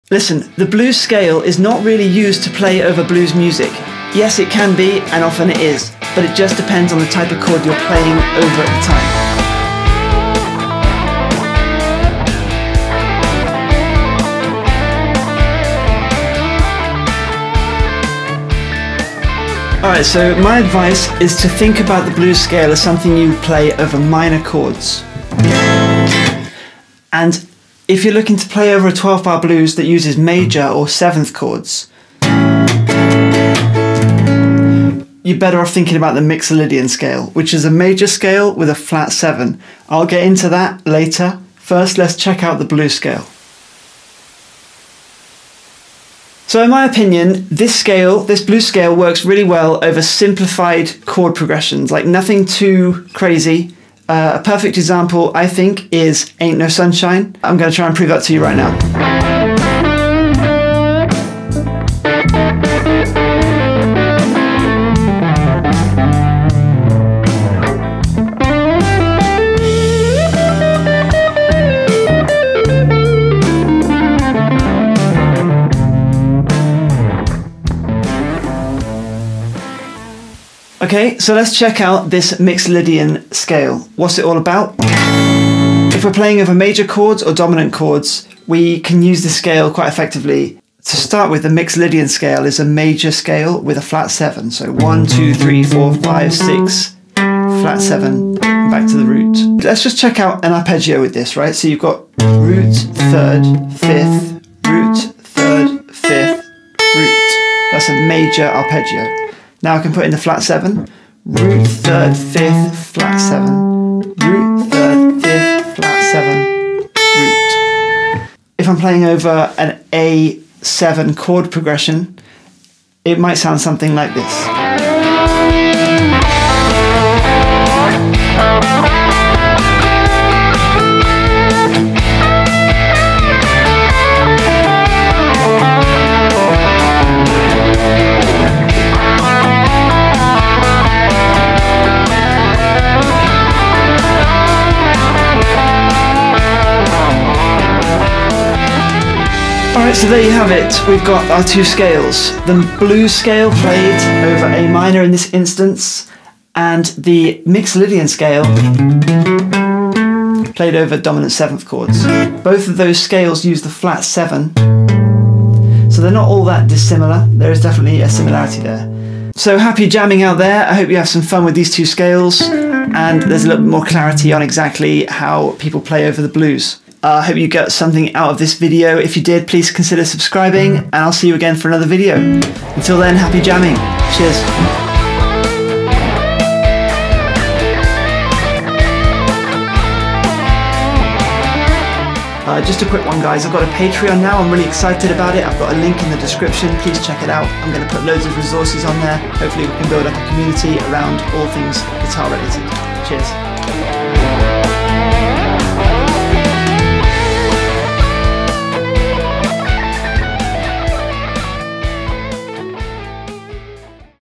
Don't be confused about The Blues! (free PDFs)
DON'T MAKE MY MISTAKE!
I was young and didn't have a teacher, so I assumed the blues scale was used when playing over all Blues music... I get it now of course, and I want to help other people understand it too...